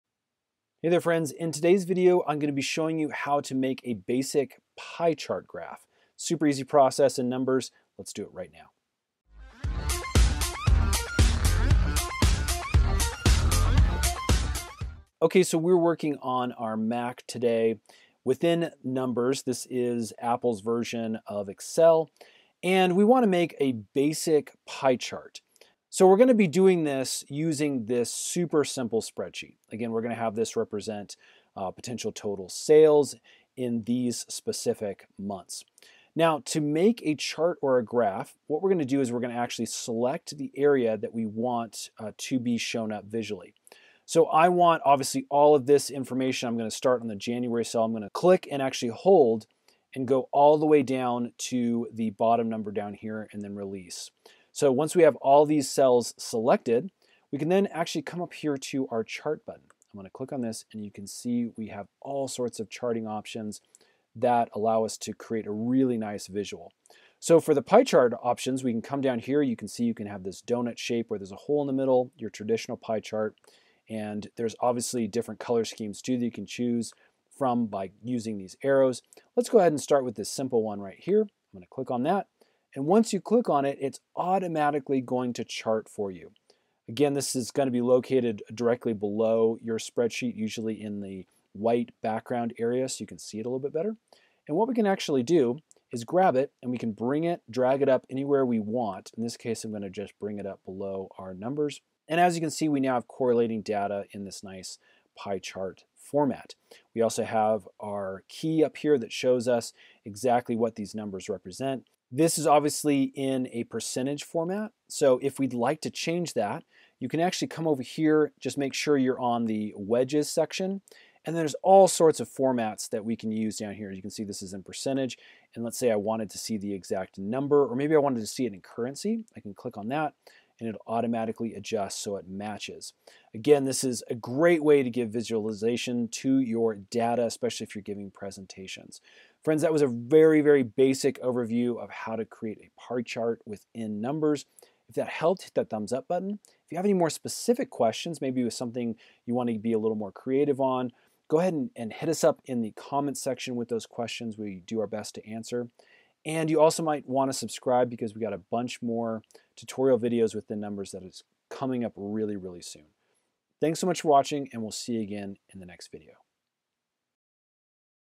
How to Make A Pie Chart in Apple Numbers
Check out today's special tech deals:

In this video, we show you how to create a pie chart in Apple Numbers. This is a great way to give visualization to your data. Pie charts can be created very simply using basic information from your spreadsheet. We walk you through the process step-by-step so you can bring pie charts into your own Apple Numbers spreadsheet.